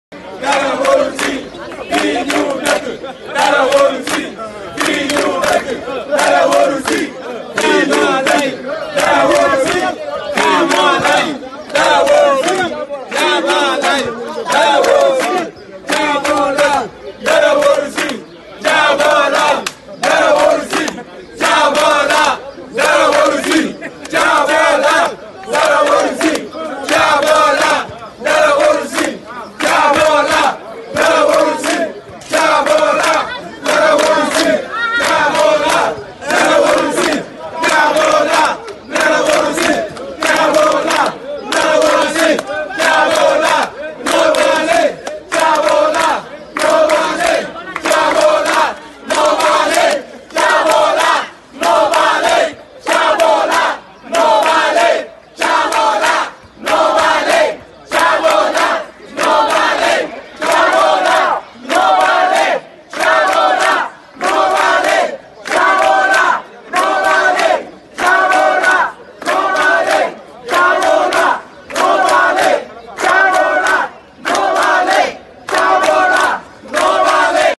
Concentración de inmigrantes en Lepe
Concentración de inmigrantes en Lepe pidiendo una solución a la situación de los asentamientos chabolistas.